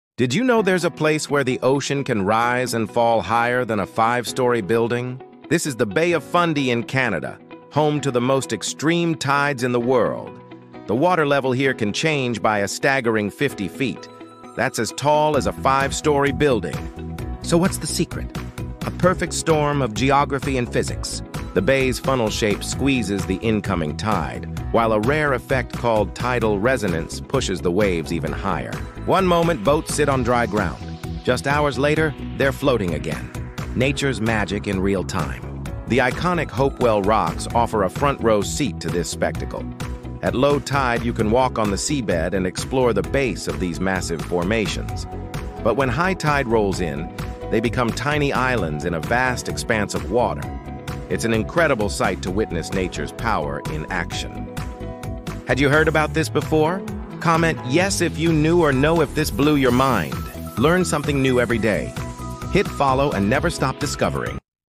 🌊 The Bay With the Highest Tides on Earth! 🌍earth
Did you know there’s a place where the ocean can rise and fall by more than 50 feet (16 meters) twice a day? 😱

Welcome to the Bay of Fundy, located between Nova Scotia and New Brunswick, Canada 🇨🇦 — a natural wonder where physics and the ocean collide.

The bay’s funnel shape forces the incoming tide into a narrow space, compressing millions of tons of seawater. Add to that a unique resonance effect, and you get the most extreme tides on Earth. 🌊

At low tide, the ocean floor is completely exposed — boats rest on mud, and kilometers of coastline appear out of nowhere. Hours later, those same boats float again as the water rushes back in. ⚓

The area is also a natural paradise: dramatic cliffs, whale sightings, ancient fossils, and the iconic Hopewell Rocks, which appear and vanish with every tide. 🪨✨

💭 Can you imagine watching the ocean literally disappear before your eyes?
Save this post for your travel bucket list — this is one of the most breathtaking places on Earth! 🌎💦
💡 Learn something new every day — hit follow and never stop discovering!